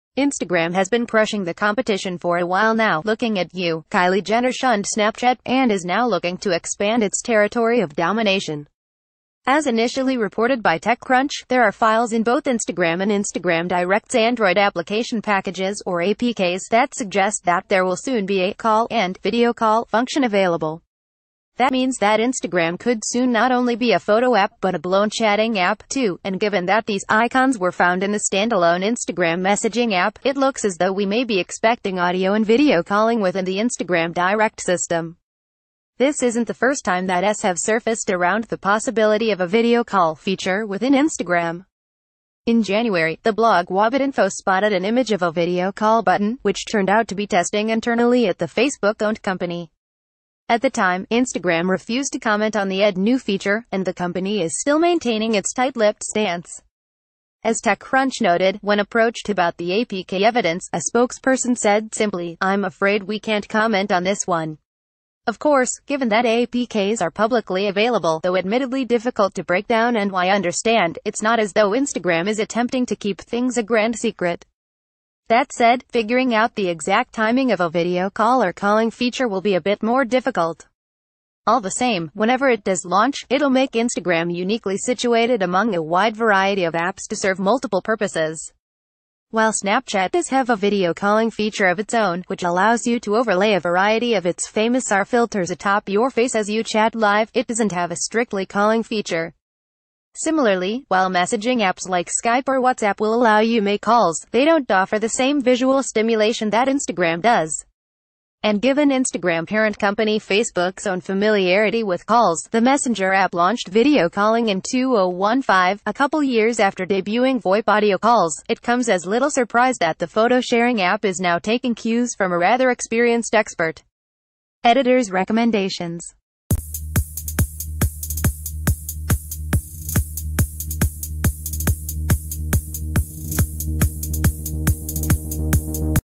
Say what? Instagram could soon be launching voice and video calling
Instagram has been crushing the competition for awhile now (looking at you, Kylie-Jenner-shunned-Snapchat), and is now looking to expand its territory of domination. As initially reported by TechCrunch, there are files in both Instagram and Instagram Direct’s Android Application Packages (or APKs) that suggest that there will soon be a “Call” and “Video Call” function available. That means that Instagram could soon not only be a photo app, but a -blown chatting app, too. And given that these icons were found in the standalone Instagram messaging app, it looks as though we may be expecting audio and video calling within the Instagram Direct system. This isn’t the first time that s have surfaced around the possibility of a video call feature within Instagram. In January, the blog WAbetainfo spotted an image of a video call button, which turned out to be testing internally at the Facebook-owned company. At the time, Instagram refused to comment on the ed new feature, and the company is st...